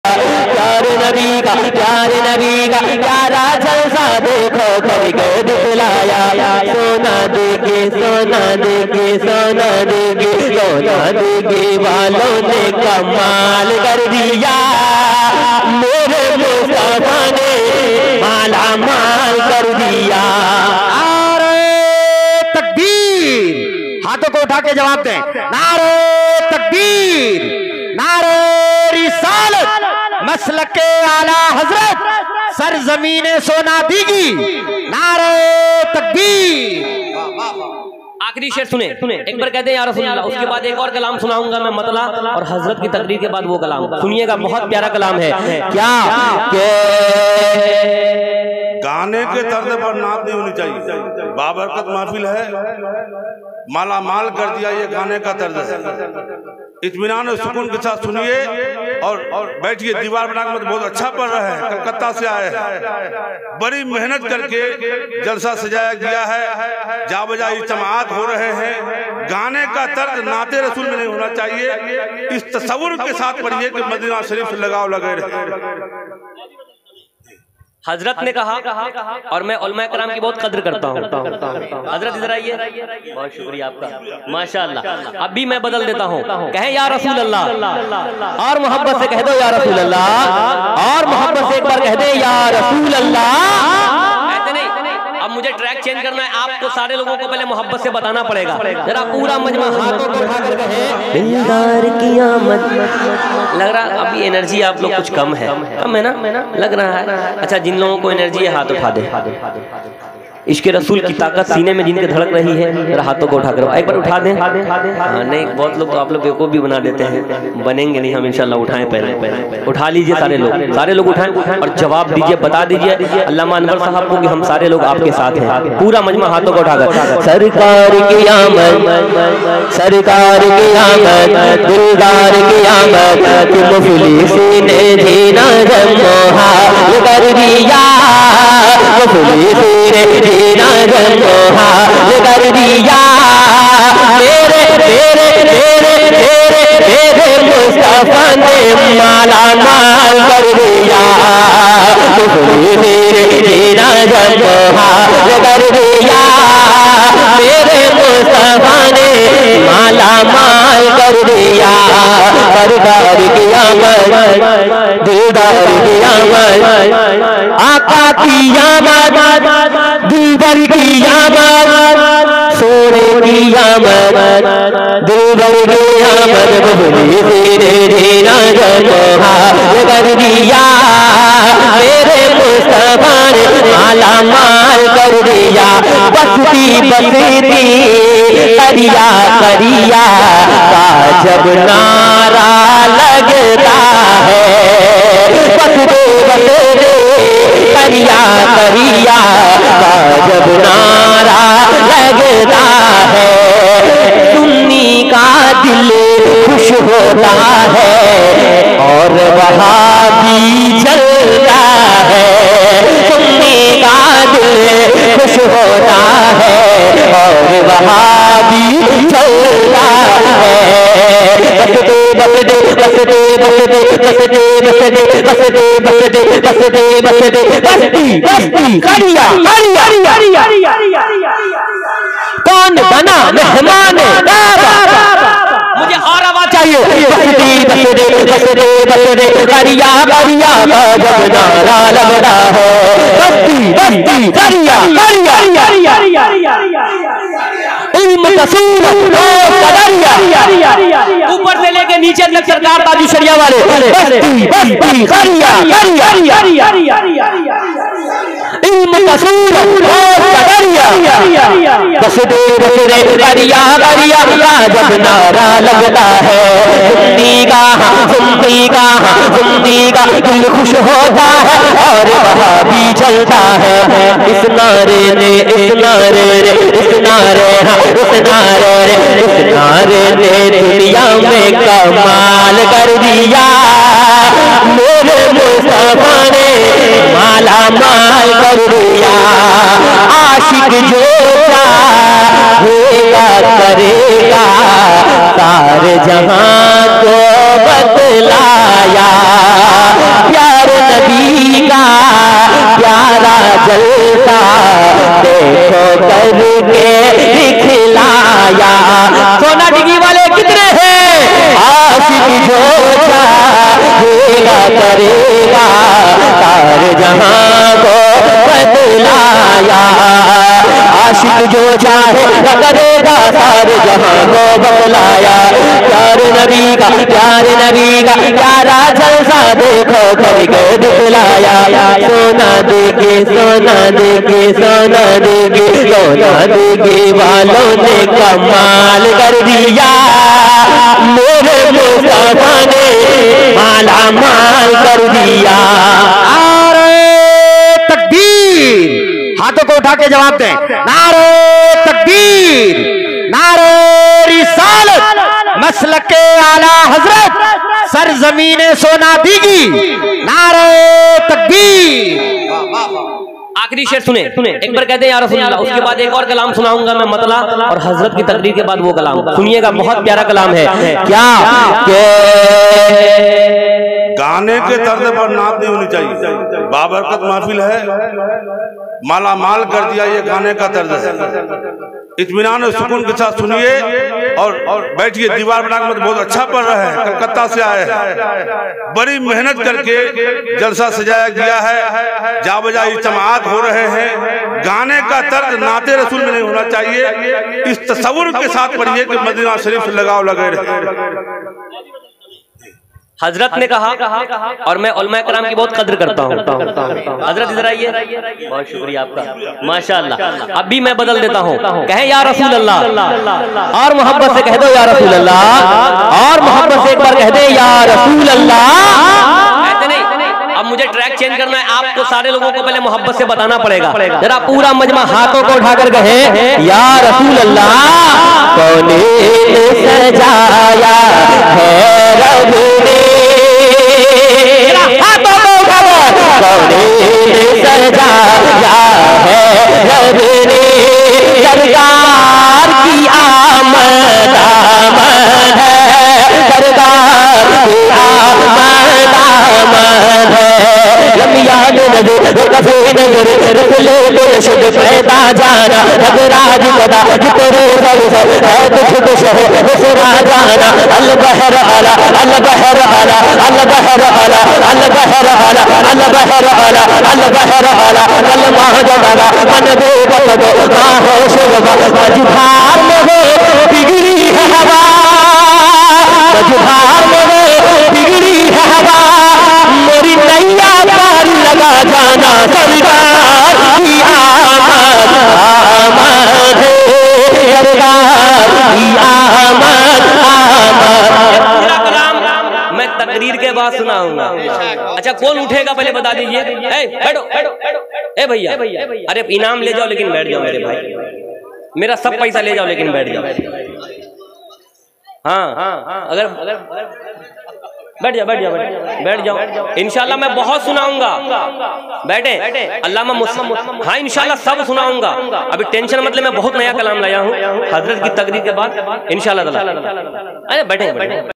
Shahbaz Raza Noori New Naat।सोना दीगी वालों ने कमाल कर दिया। Shahbaz Raza Kalkattavi। जलसा सोना दीगी
Shahbaz Raza Noori New Naat।सोना दीगी वालों ने कमाल कर दिया। Shahbaz Raza Kalkattavi। जलसा सोना दिया

जलसा सोना दीगी बहादुरगंज किशनगंज#
सोना दीगी वालों ने कमाल कर दिया#

About Video :- Shahbaz Raza Noori Kalkattavi